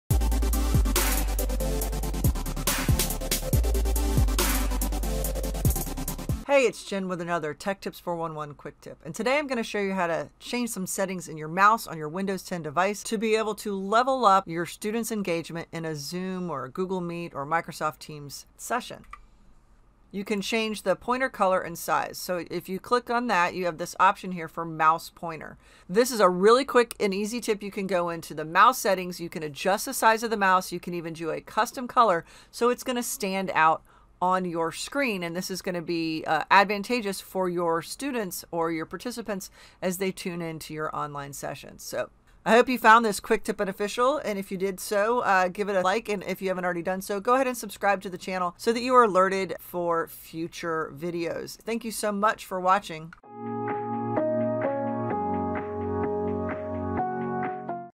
How to Customize Windows 10 Cursor | Quick Tip
How to customize your Windows 10 cursor for easier viewing during online classes, meetings, and screen recordings.